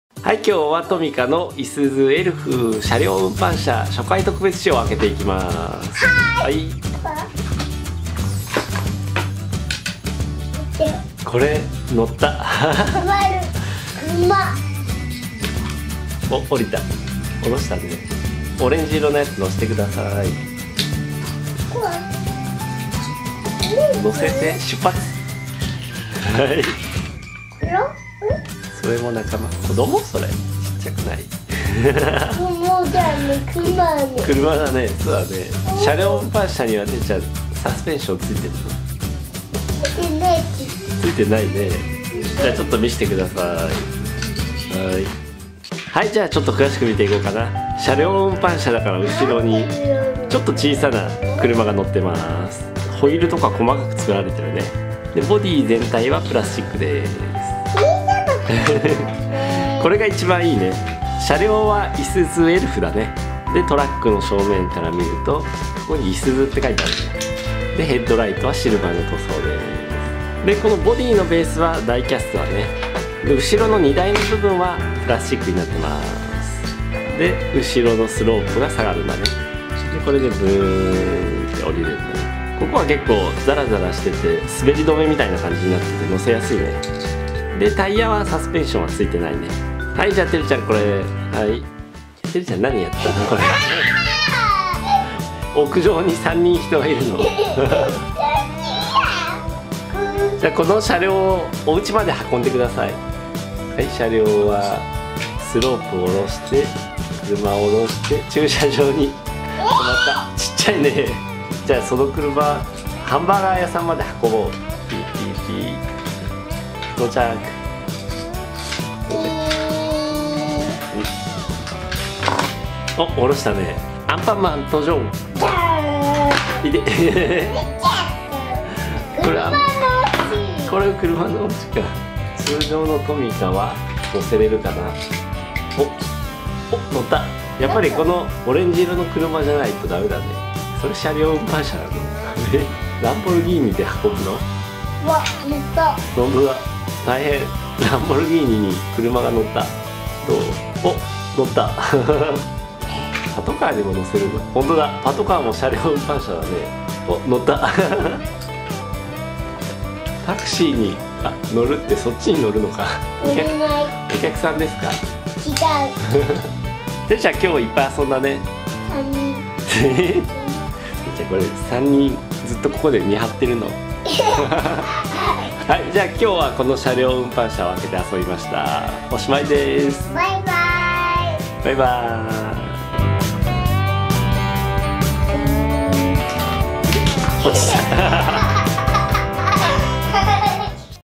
【トミカ】 トミカタウン に仲間がふえたよ いすゞ エルフ 車両運搬車 初回特別仕様
いすゞ エルフ 車両運搬車 初回特別仕様を開封して、トミカタウンで遊びました。

★Amazon 商品ページ
トミカ No.60 いすゞ エルフ 車両運搬車 (初回特別仕様)